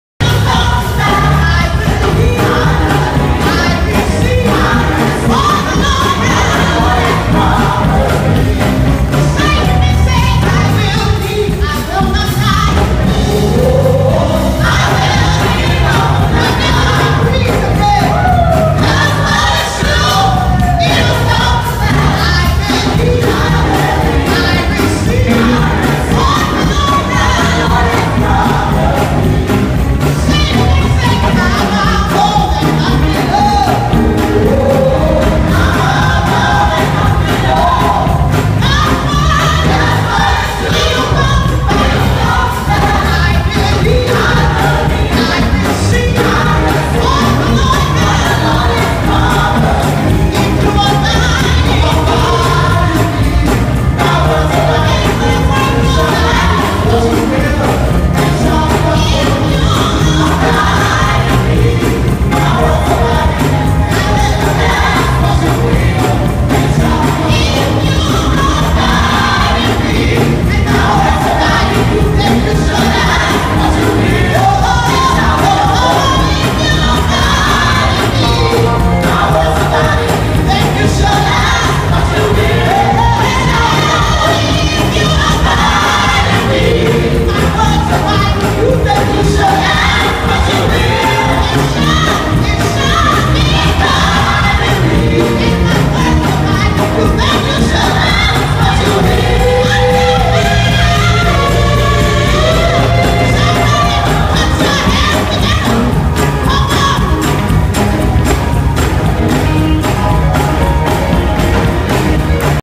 CoGNigerianStyleWorship
Church of God Nigeria style of worship in Benin City. The lead singer is Rhose Avwomakpa. Some of the words (I think) are "I believe, I recieve all the Lord has promised me. I will live; I will not die. God's Word is true. If You abide in me I will survive in You."